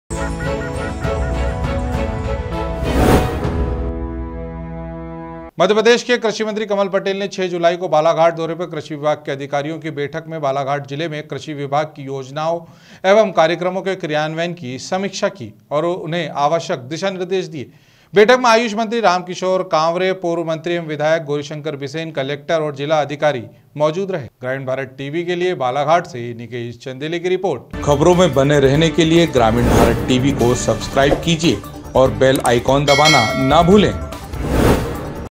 Balaghat में कृषि मंत्री पटेल ने समीक्षा बैठक में कहा नकली खाद, बीज, दवा बेचने वालों पर हो कार्यवाही
कृषि मंत्री कमल पटेल ने ली कृषि अधिकारियों की बैठक
नकली खाद, बीज, दवा बेचने वालों पर हो कार्यवाही : मंत्री
किसानों के साथ धोखाधड़ी बर्दाश्त नहीं की जायेगी : मंत्री